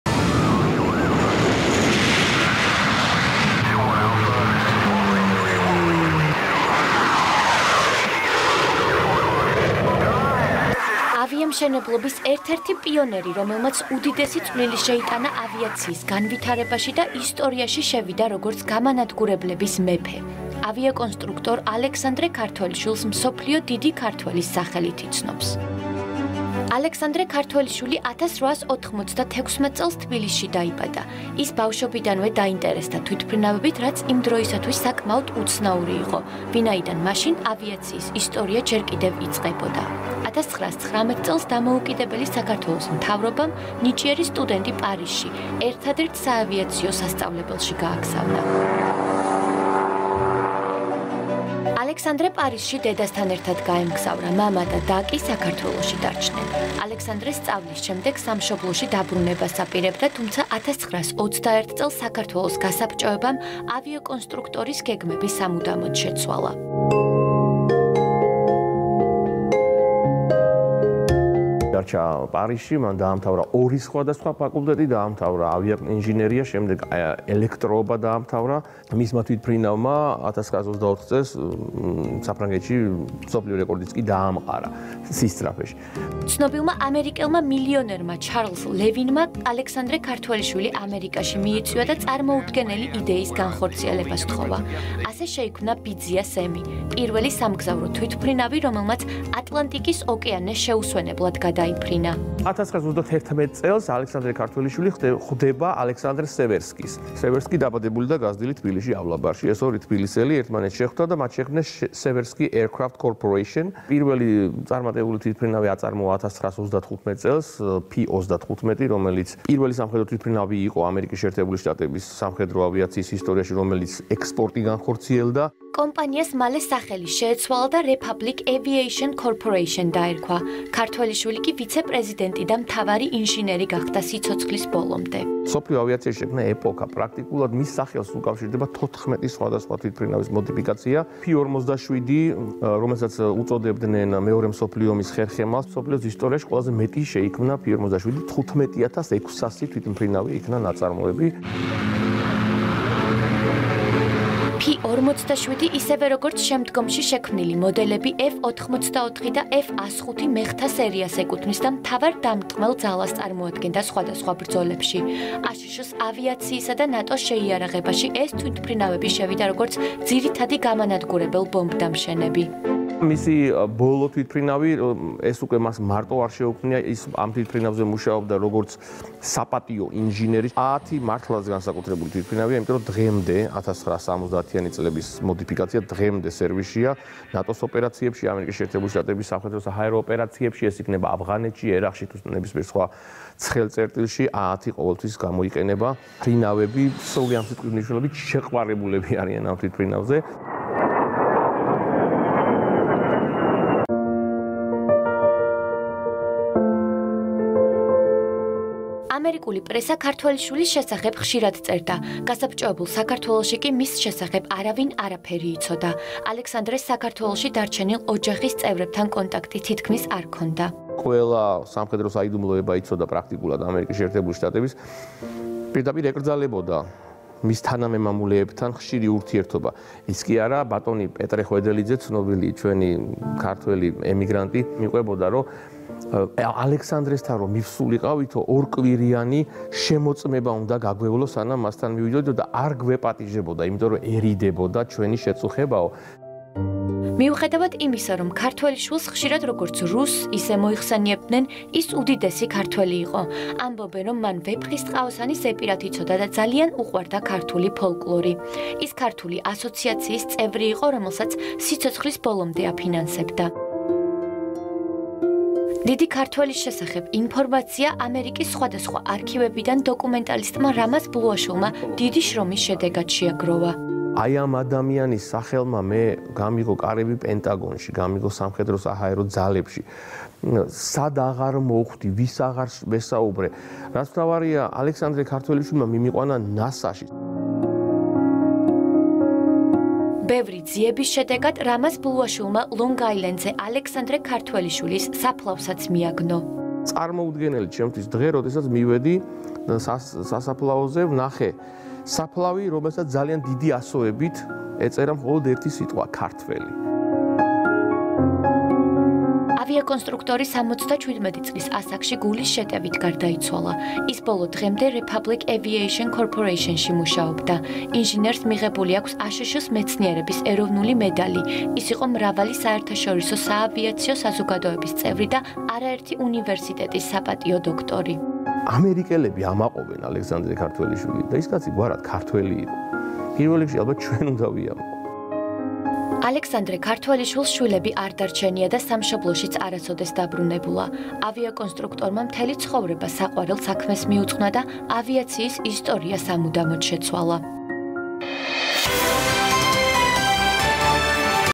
ალექსანდრე ქართველიშვილი - ამერიკაში მოღვაწე ცნობილი ავიაკონსტრუქტორი
ამერიკაში მოღვაწე  ქართველი ავიაკონსტრუქტორი, რომელმაც ისტორიული კვალი დატოვა ავიამშენებლობაში.